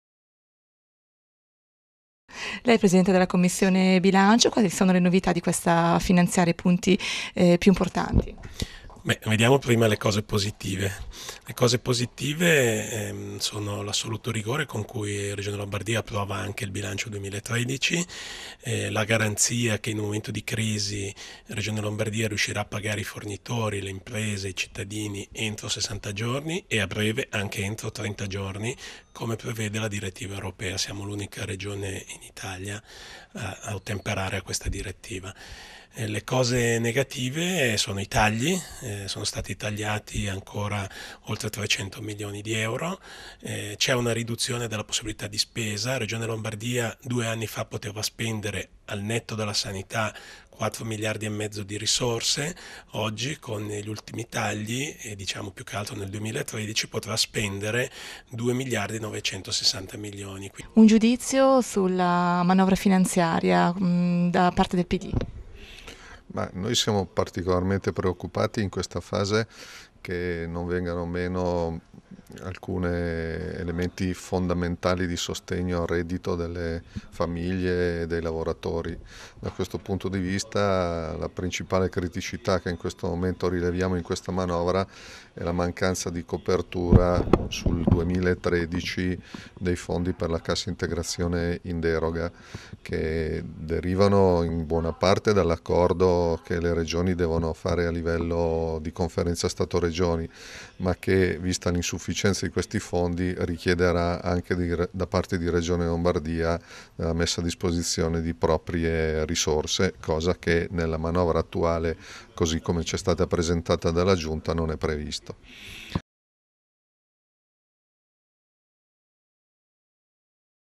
REGIONE LOMBARDIA, FINANZIARIA IN COMMISSIONE.mpg
La manovra finanziaria della Regione Lombardia viene discussa in Commissione Bilancio. Ne parla il presidente Ugo Parolo (Lega Nord).